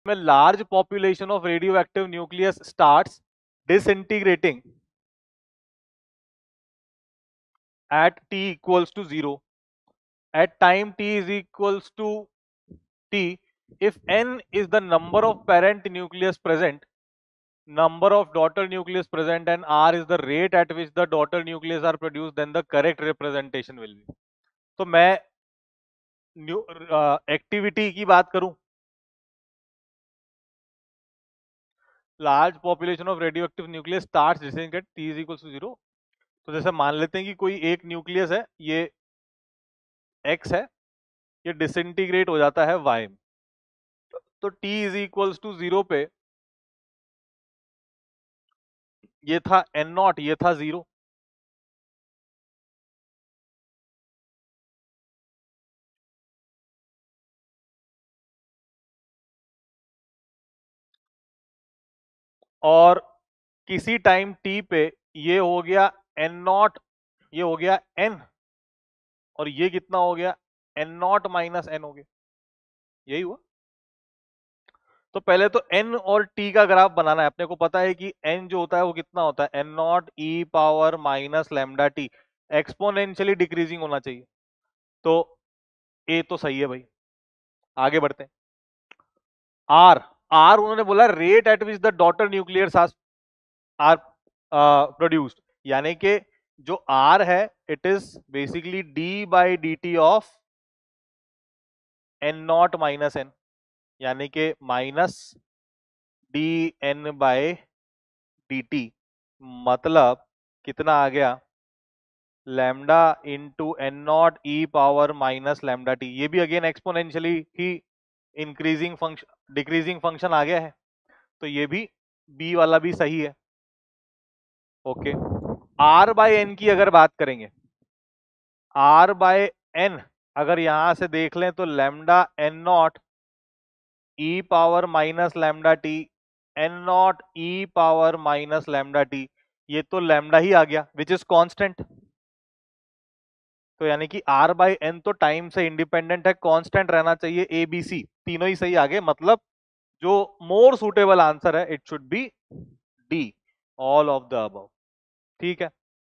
A large population of radioactive nucleus starts disintegrating at t = 0. At time t, if N = numb....
A large population of radioactive nucleus starts disintegrating at t = 0. At time t, if N = number of parent nuclei present, D = the number of daughter nuclei present and R = rate at which the daughter nuclei are produced, then the correct representation will be 📲PW App